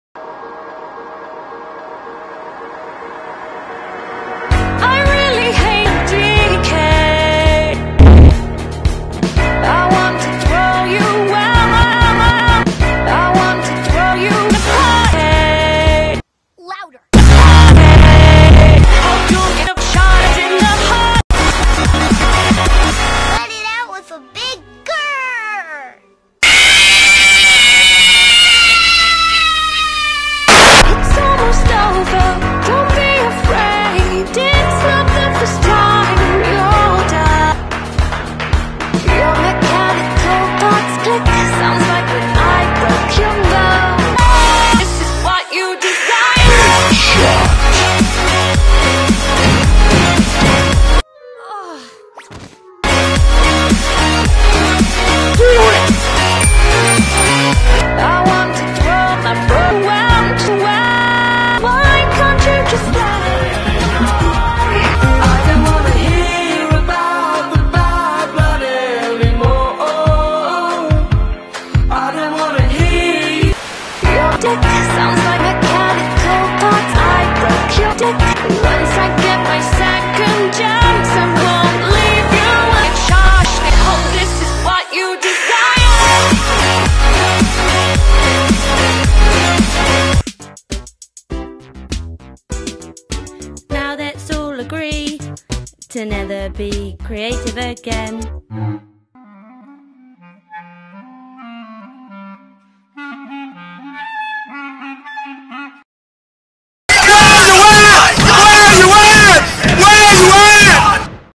YTP - Freddy Fazbear Invades An SJW's Personal Space
I looked back at my upload date for My Not So Little YTP of Ponies as Humans. That YTP is from September 2013. Also, I heard the word decay which sounded like DK, so I decided to make a YTP of this song.